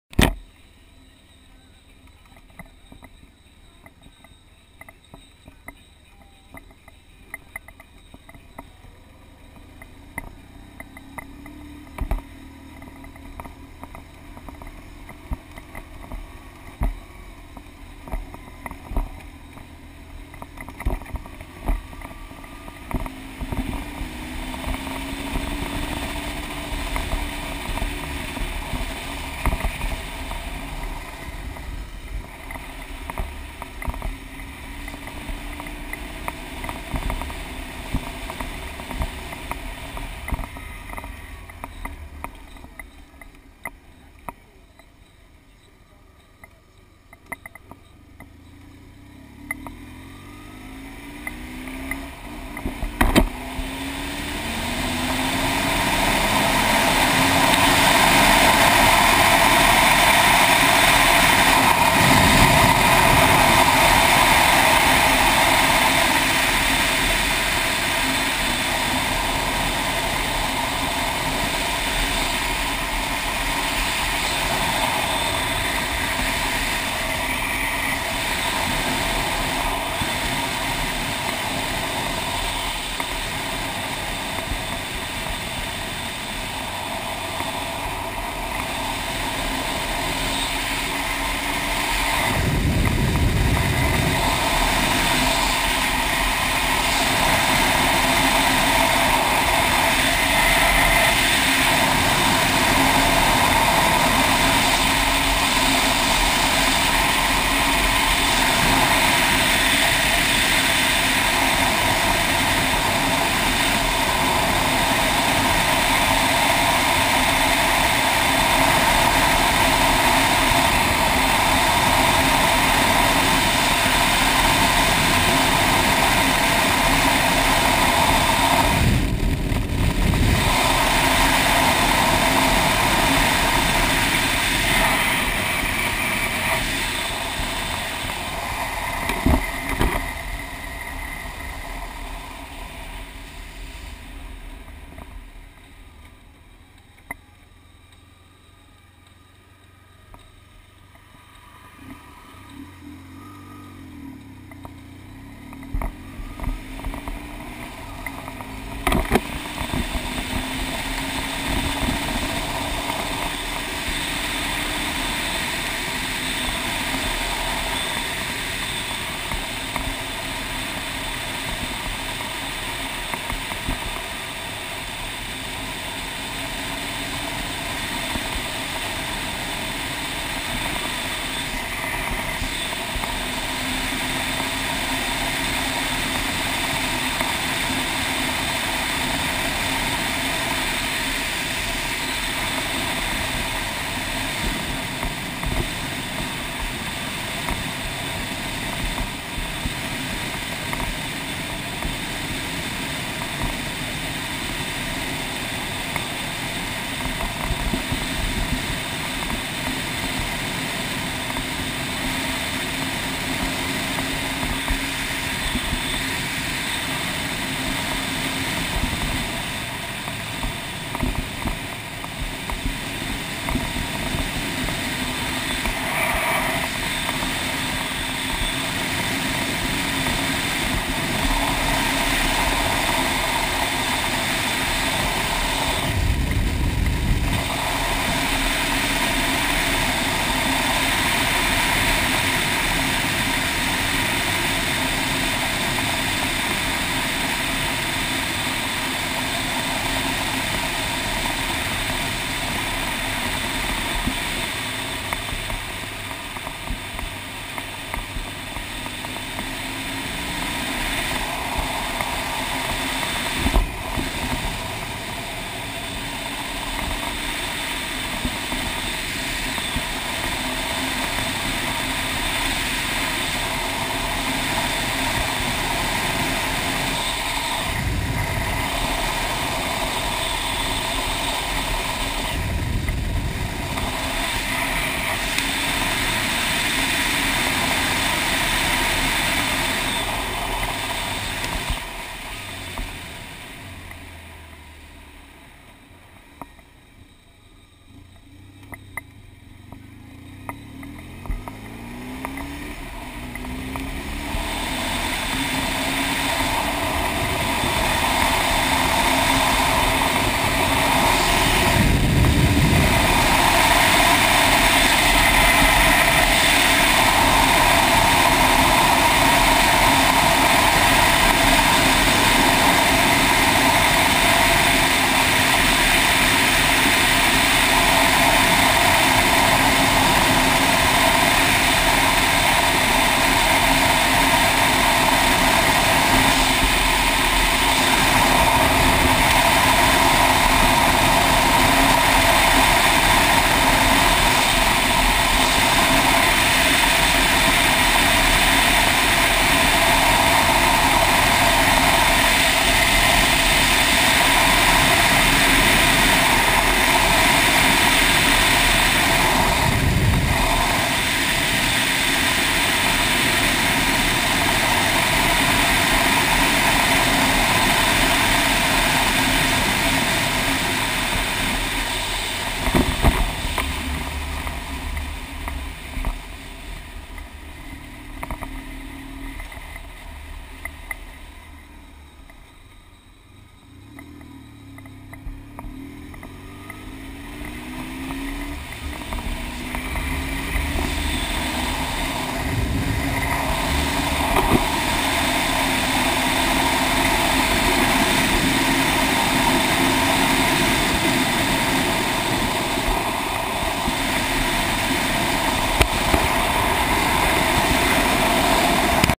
Go Pro Test
Local dirt roads, with a BIRD STRIKE around the 4 minute mark.